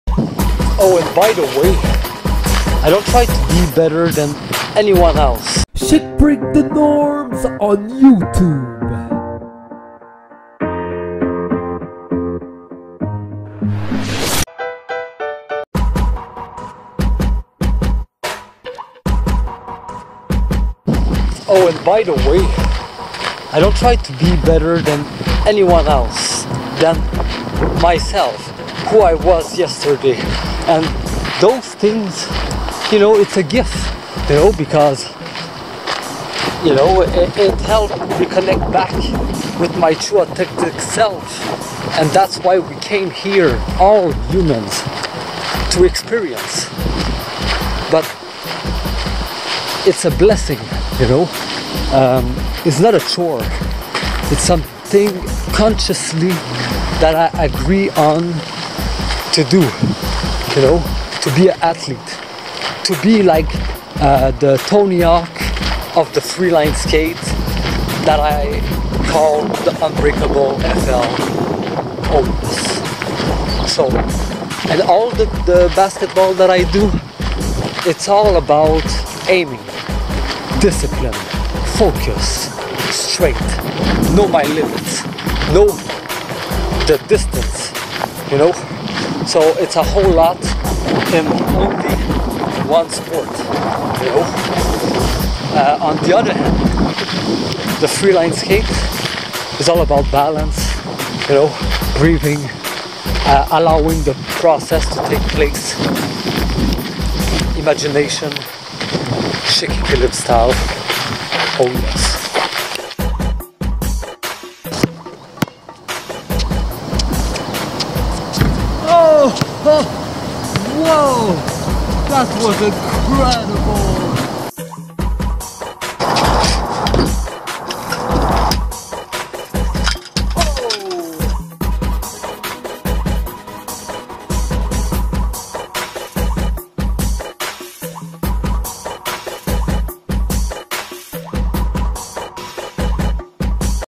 🧙‍♂️Chic Topic 🧶Evergreen⭐️ - Better then who i was Yesterday
🧙‍♂️This was  a vision that I have at the moment to talk about  certain thing for you to know more about me and what I stand for. To share my journey and be able to inspired you to take over your  own life with the right set of tools. More on that topic and others for you to know more and be able to learn from the best asset that I gain so for on my journey of manifesting my wildest dreams. Enjoy and wish you love 💛 & 🔦 light to all of you. Together we can thrive and  create a better world.